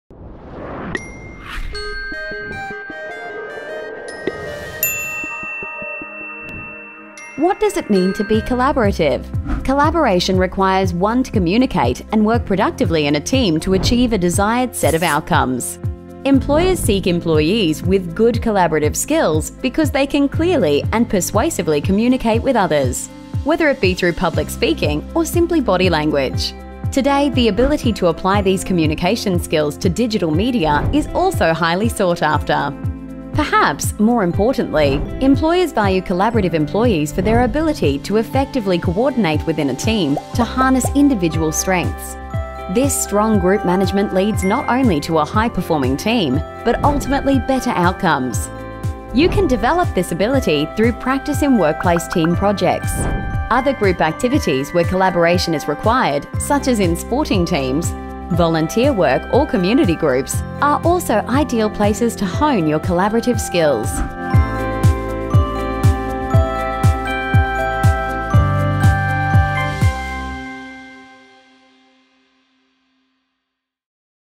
Launch U: Collaborative
Being collaborative means you are able to communicate and work in teams effectively and productively to achieve desired outcomes. There are two important aspects of collaboration: Firstly, communication is the ability to clearly articulate an idea or opinion into a persuasive message through a variety of contexts and media. Secondly, collaboration involves being able to coordinate professionally with others to effectively and productively achieve desired outcomes.